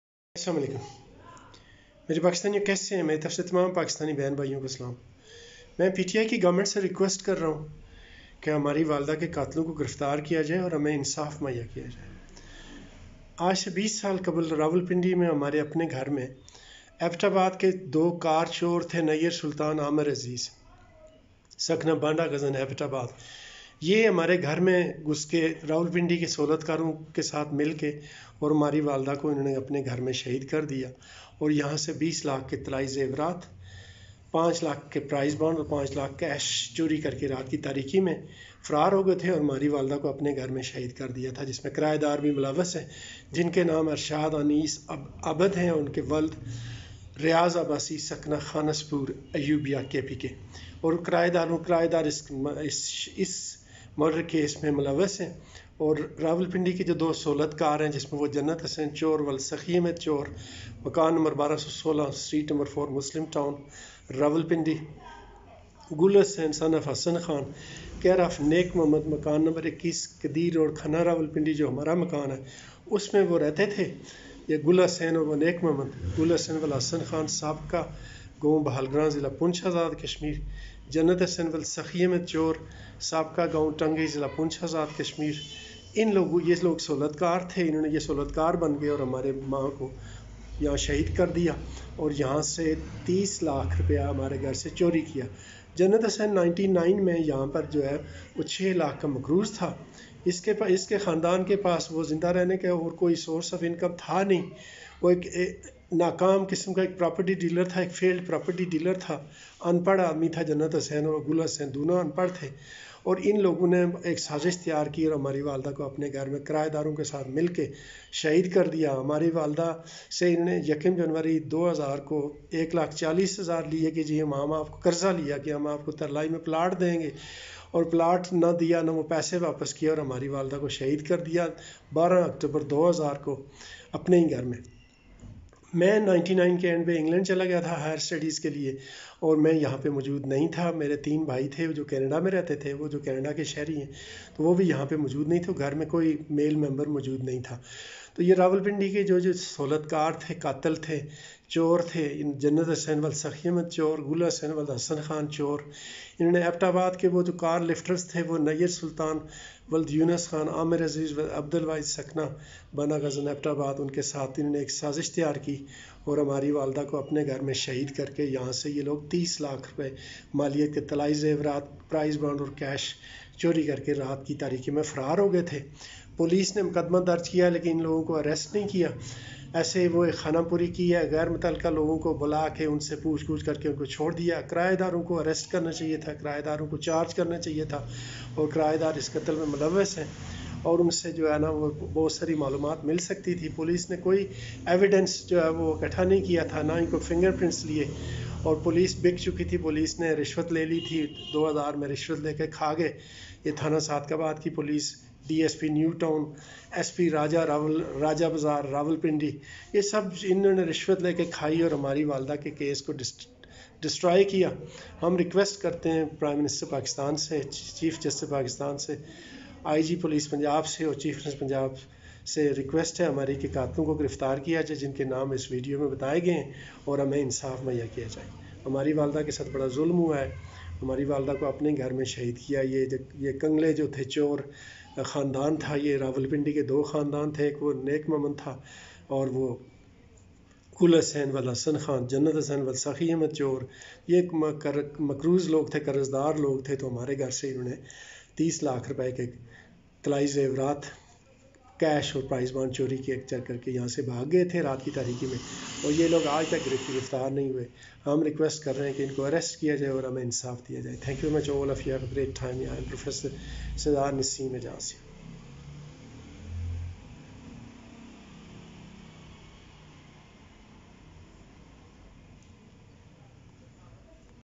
car lifters criminals Nayyar sultan and Amir Aziz Banda ghazan Abbottabad looted 3 millions from
Car lifters criminals Nayyar sultan and Amir Aziz Banda ghazan Abbottabad looted 3 millions from our home and killed our innocent mother. Janat Hussain 1216 Muslim town Rwp, Gul Hussain 21 qadeer Road Khanna Rwp looted 3 millions from our home. Requesting for justice and arrest criminals Nayyar sultan and Amir Aziz Banda ghazan Abbottabad